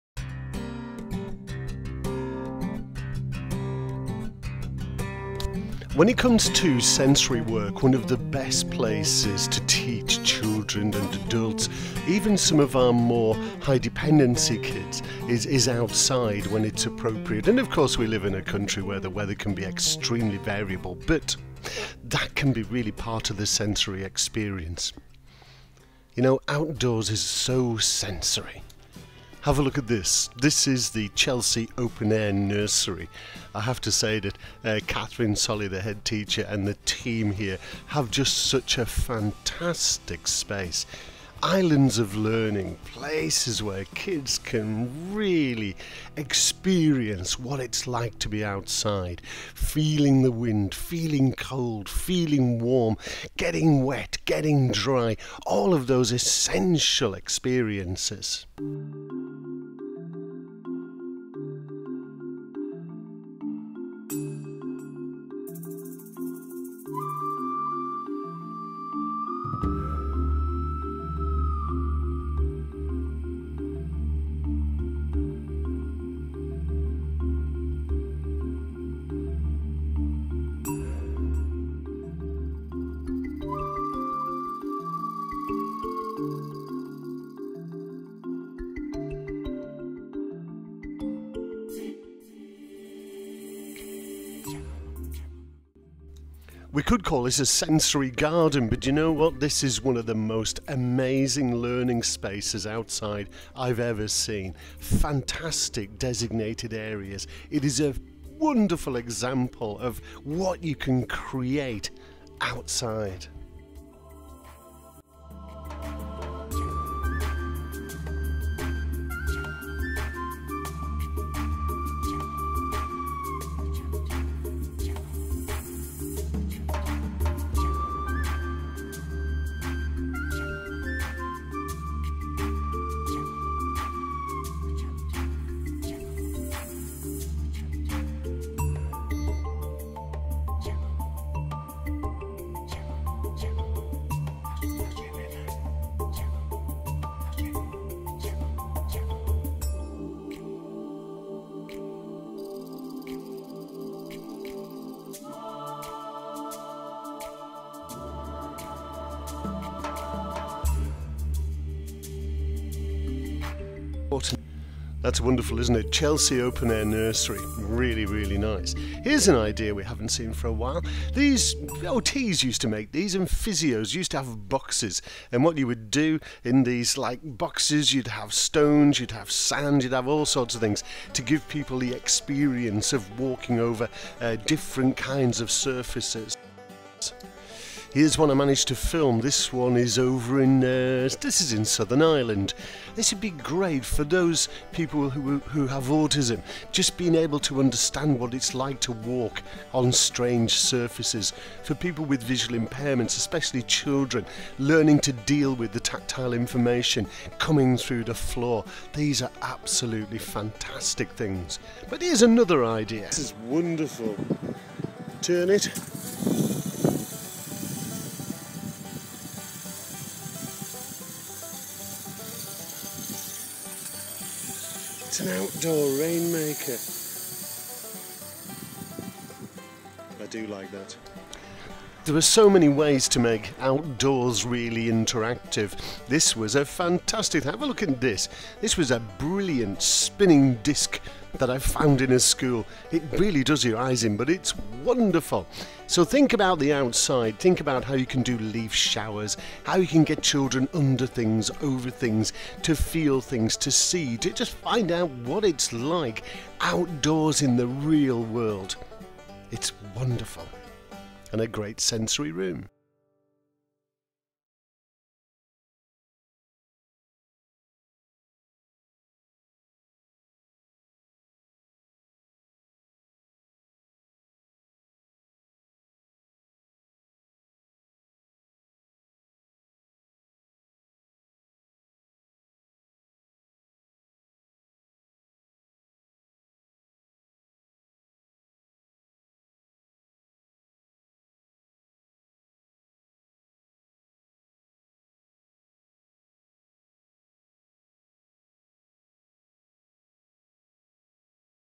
Chelsea Open Air Nursery.mp4
This is part of the video course notes for the 'Back to the Classroom' course from Hirstwood Training'. The nursery in the film is wonderful and a little oasis of calm in the big city. A credit to Kathryn Solly the Head and her wonderful team, a fantastic example of outdoor learning.